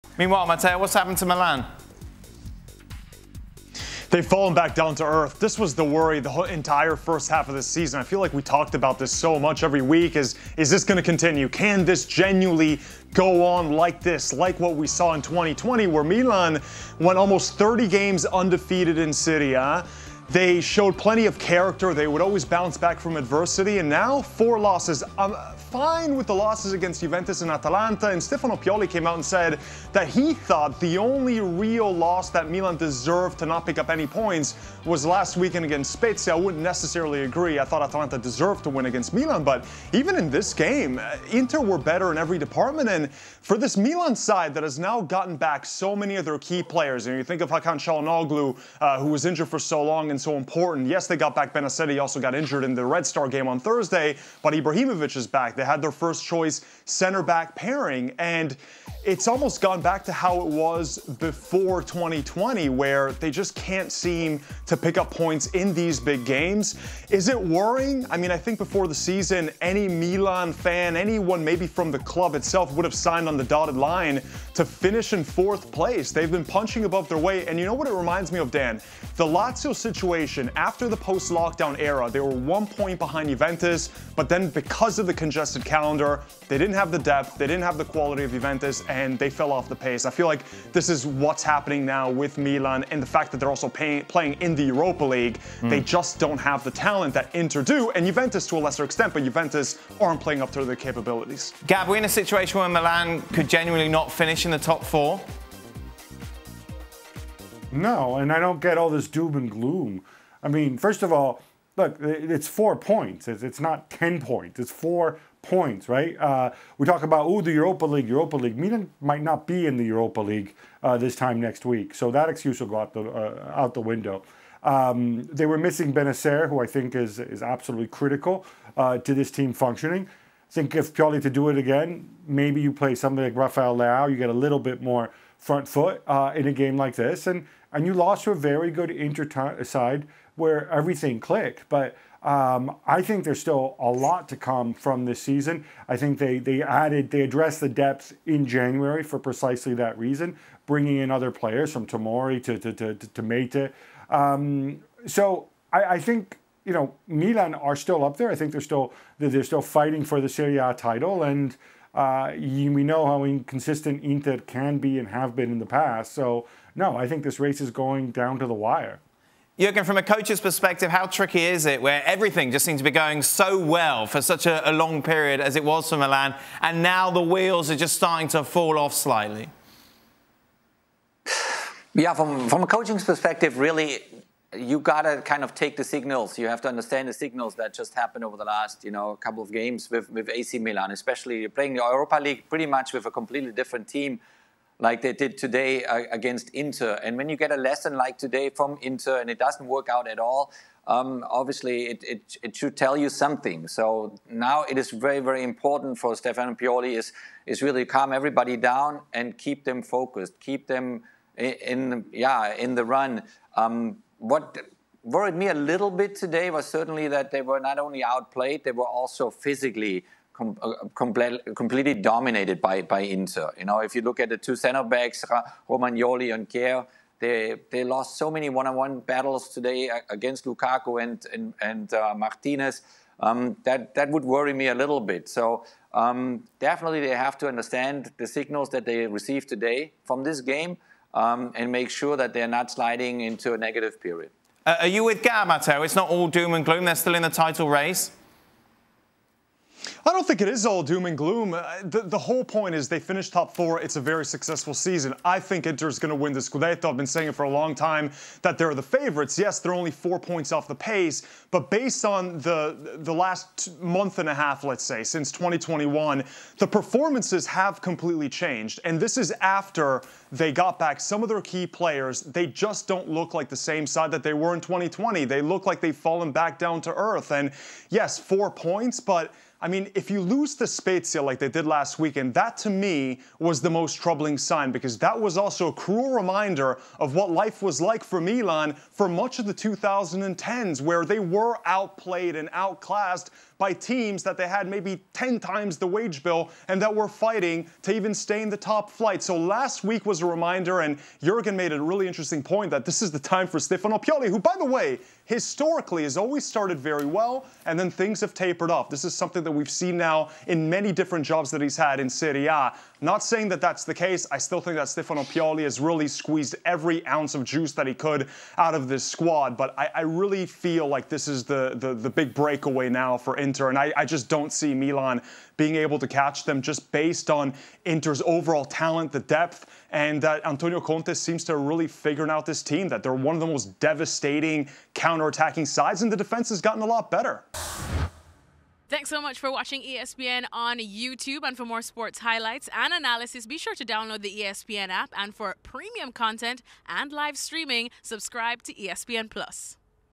Zlatan Ibrahimovic & AC Milan have come back down to Earth after Inter defeat - Bonetti | ESPN FC
Matteo Bonetti, Gab Marcotti and Jurgen Klinsmann weigh in on the state of AC Milan following back-to-back Serie A losses to Spezia and rivals Inter Milan. Bonetti believes Milan were "punching about their weight" and were due for a downturn in form. Marcotti contends that Milan are still very much in the title race and believes it could come to the wire for the Scudetto this season.